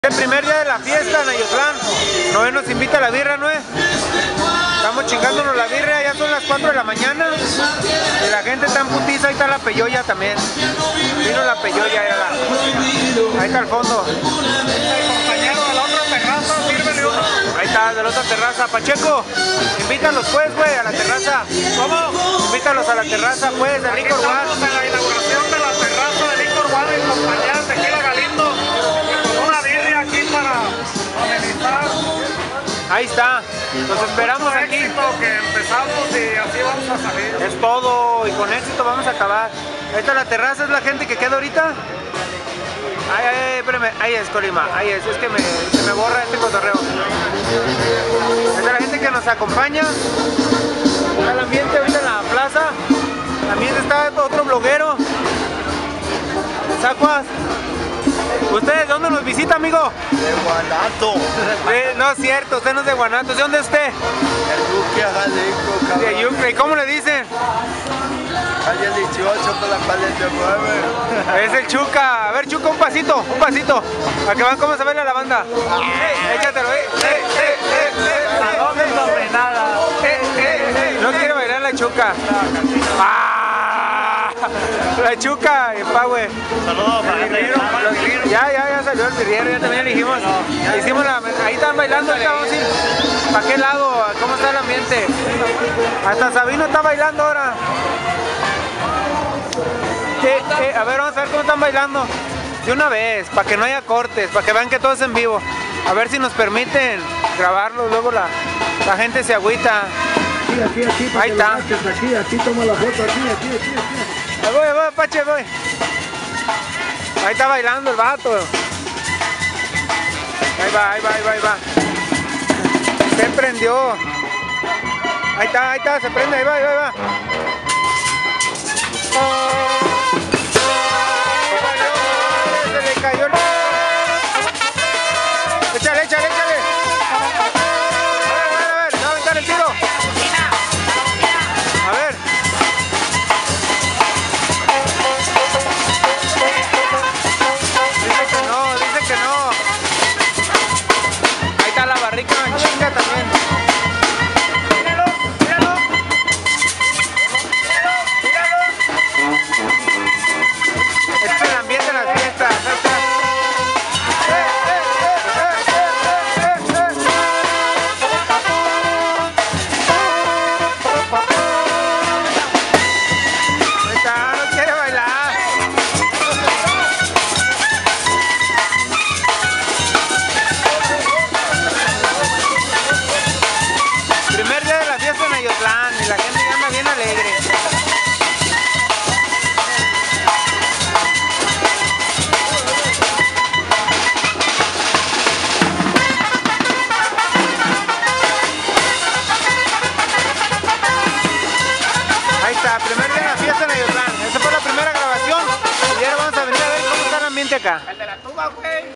Primer Día de Fiestas en Ayotlán 2013
Licor One nos invito una buena birria de chivo que preparo el amigo Noe que la verdad esta muy rica, el vende sobre la carreta en la Hermita colonia de Ayotlan, bueno en el vídeo también se ve un grupo de amigos que están a todo con su banda y unas barricas de tequila y bien los invitamos a las fiestas de Ayotlan